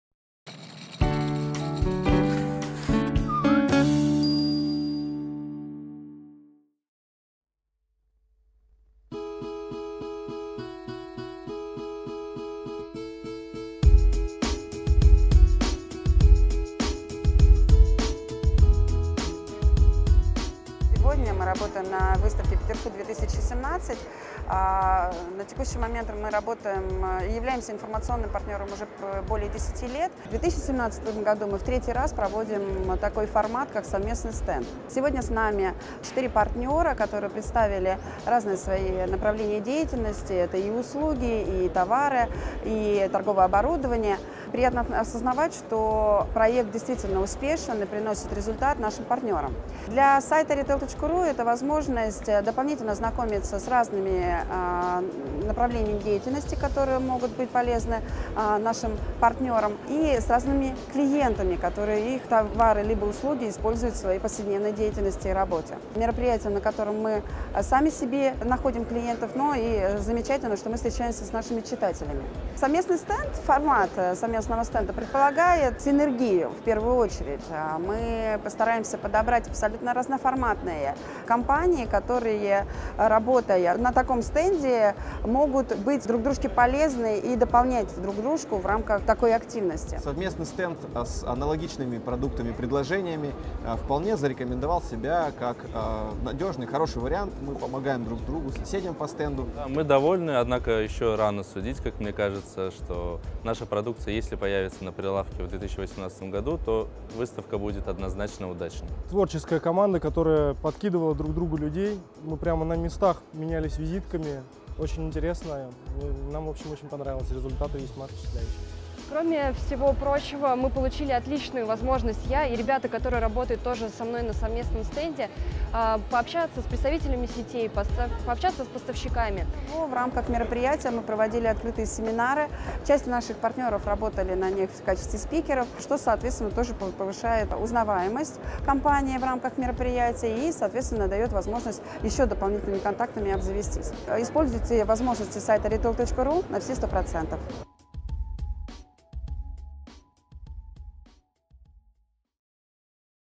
Совместный стенд Retail.ru на Peterfood 2017
Участники стенда:
"1С-Товары" - систематизация производства
"Интерволга" - веб-интергация
"ЭКО-1" - холодильное оборудование
"DEP" - мороженое из Казахстана